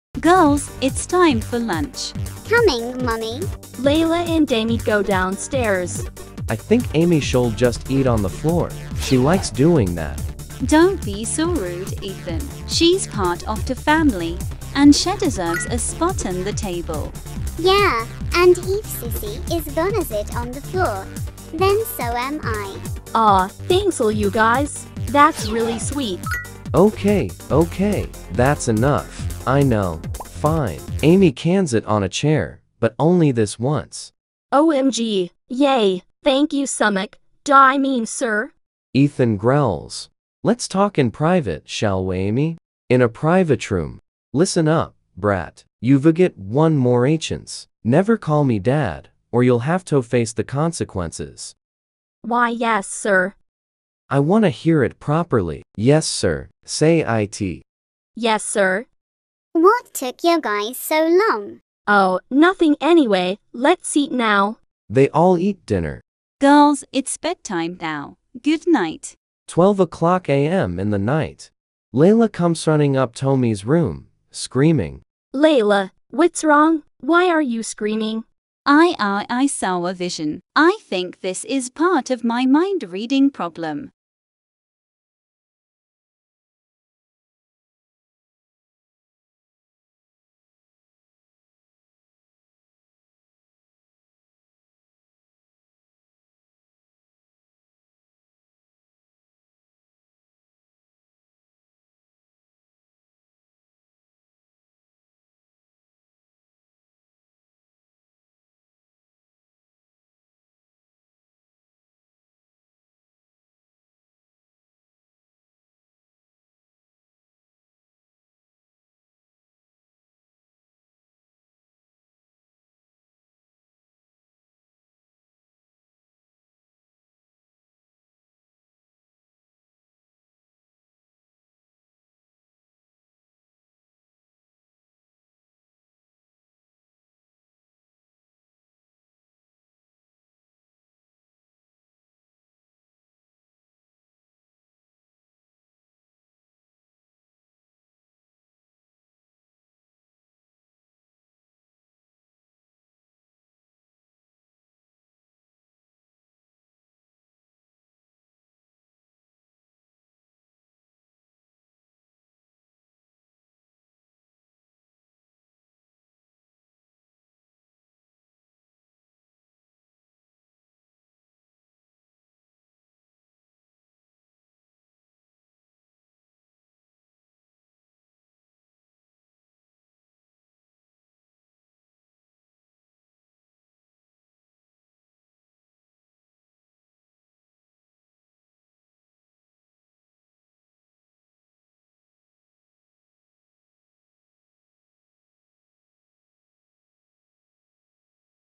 Text To Speech Story | The Major Secret | Part 2 | 💗VALENTINES DAY SPECIAL 💗 | Sister + Mom Power!
You’re beautiful 🤗

What should I do for my next video?

Hello party people 😃
Welcome back to another one of my videos! ✨
Today is a ❤️Valentines Day❤️ SPECIAL Text To Speech!

This one is based on the part where the Mom and the younger sister stands up for the abused sister as she gets abused by he ☹️abusive☹️ father!
Abuse/abusive/abused word count: 3 (😱A whopping THREE)

-LunaPlayz- Out!